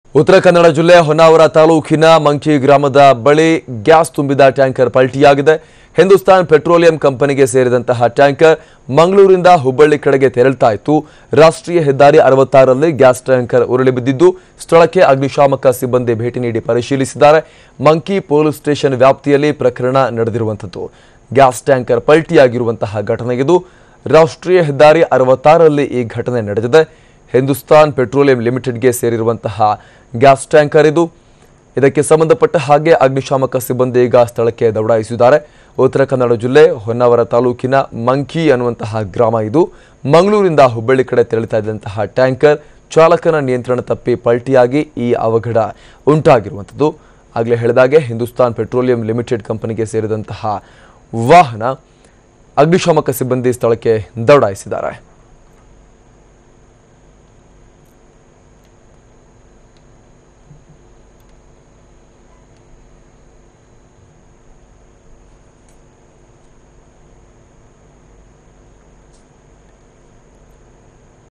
Gas Tanker Topples in Honnavar, Karwar
TV9 News: Gas Tanker Topples in Honnavar, Karwar..,

In an all too common incident that seems to happen in Uttara Kannada district, a gas tanker fell to its side. This accident was reported from Manki village in Honnavara taluk.

The tanker was bound to Hubballi from Mangaluru, and belonged to Hindustan Petroleum company. The accident took place on National Highway 66.

Firefighters arrived at the spot to ensure no leakage or similar hazards arose.